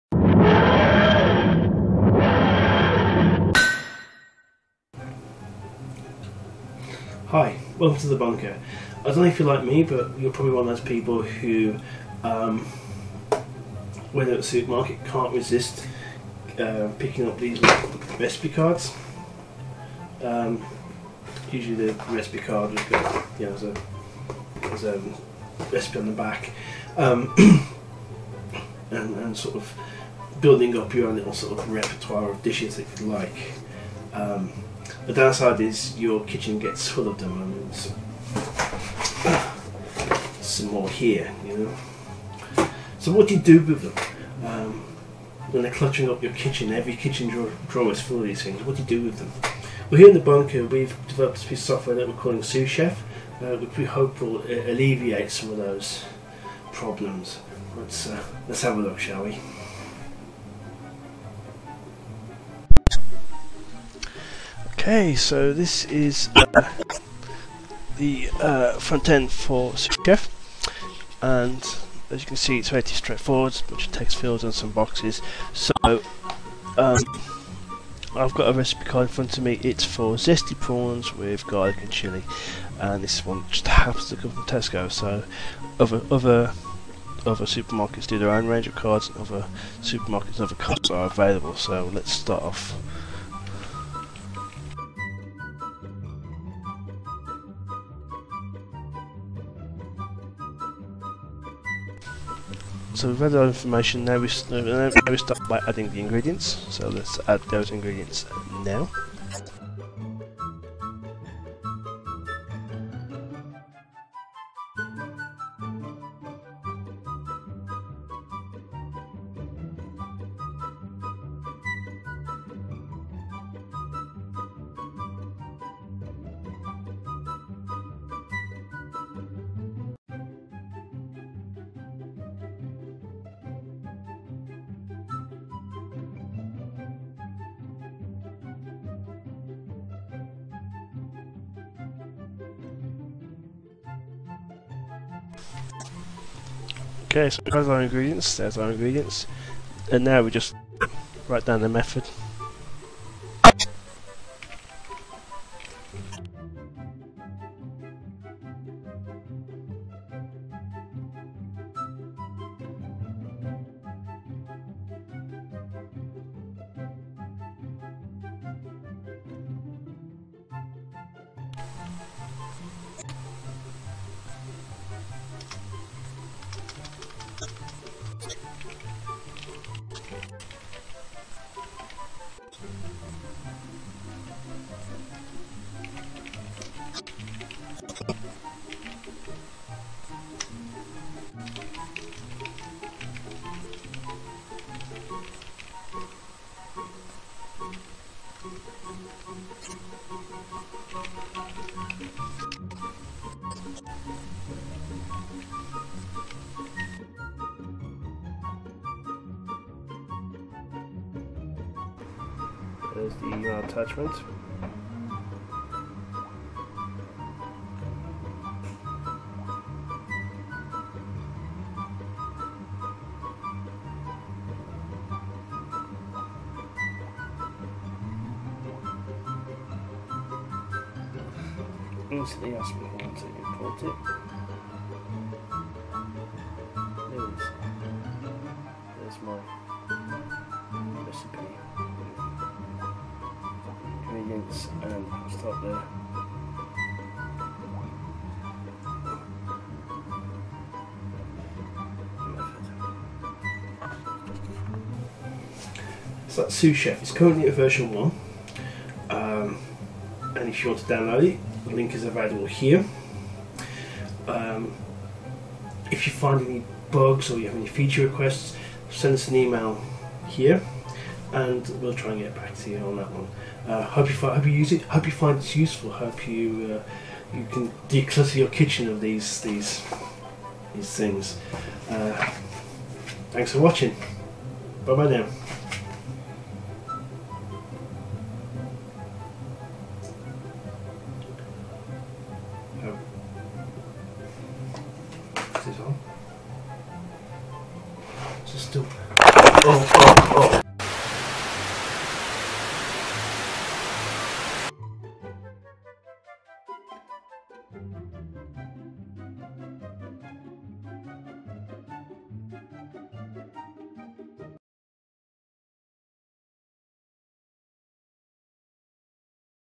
souschef
video showing SousChef. a tool for transcribing recipes for android app BITES